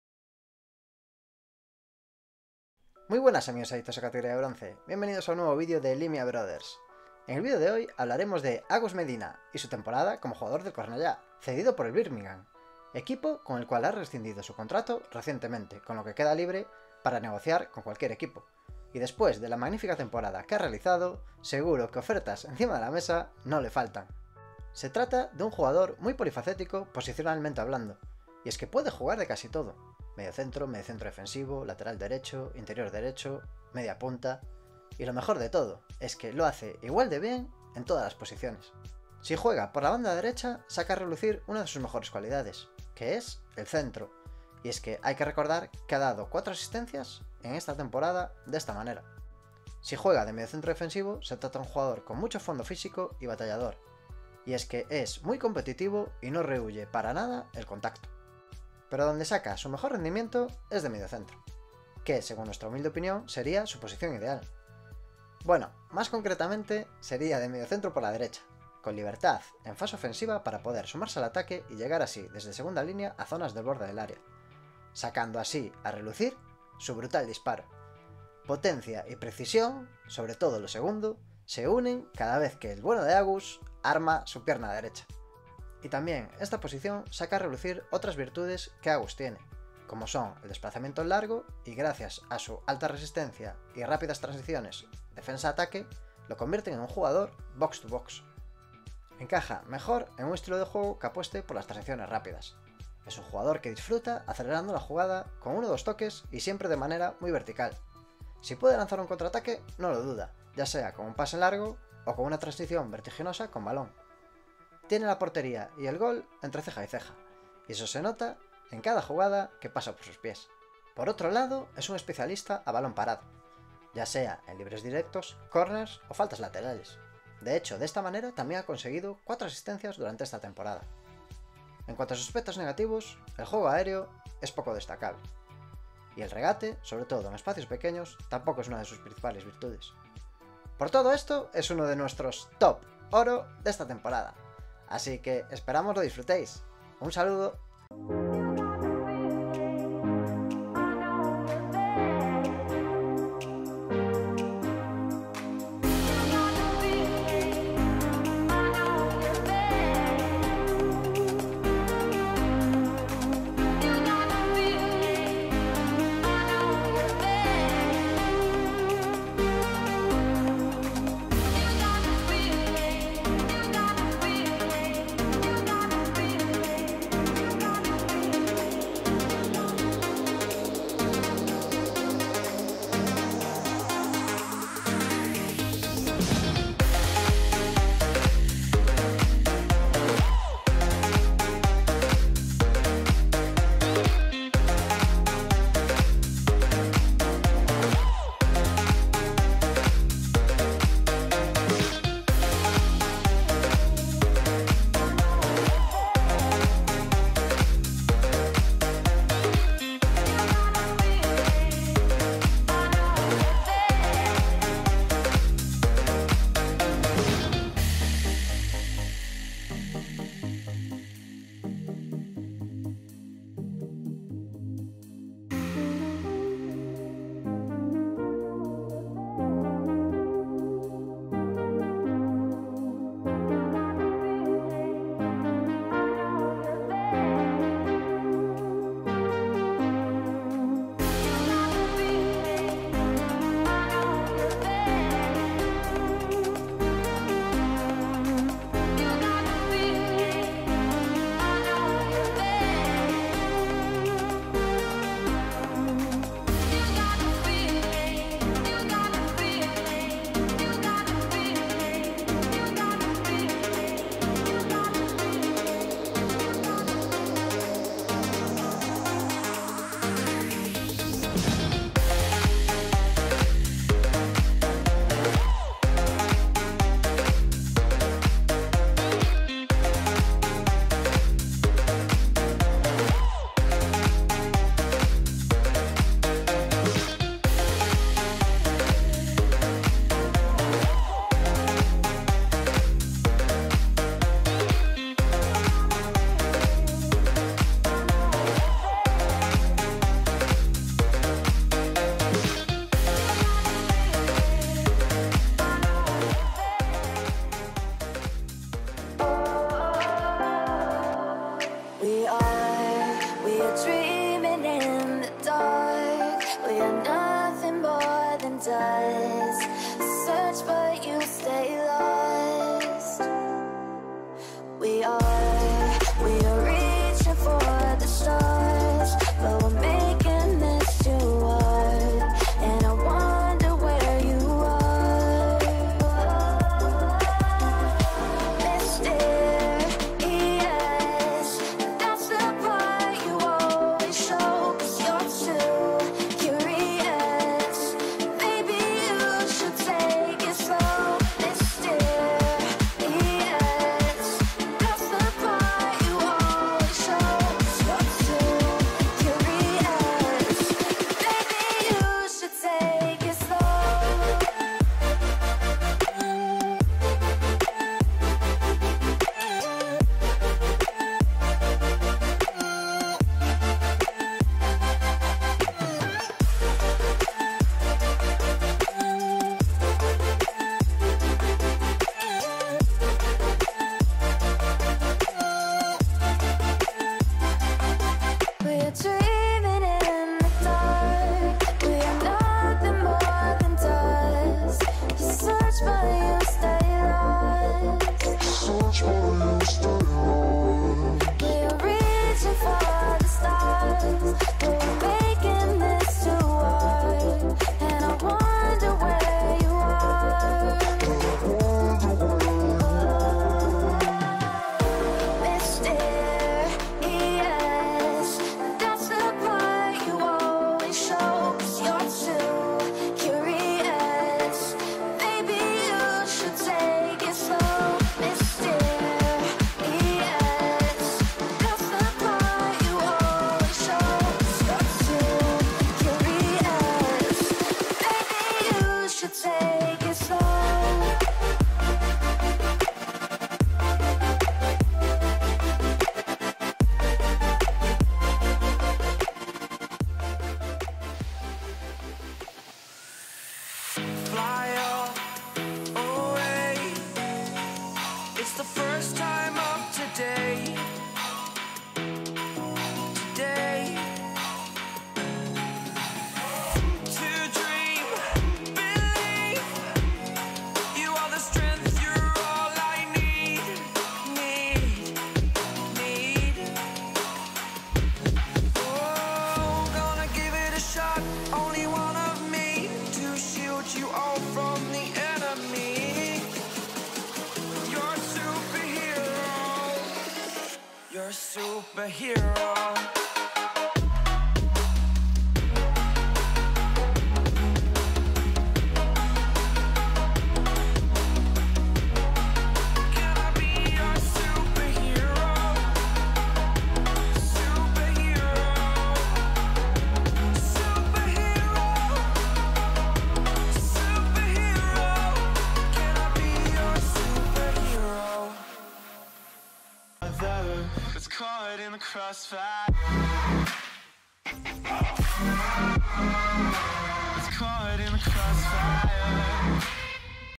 Agus Medina - Cornellá - 2020/21 - Gold TOP player - highlights (Scouting Segunda B)
New video that I hope you enjoy it of the best highlights & skills that Agus Medina has made during 2020/21 season as a player of Cornellá.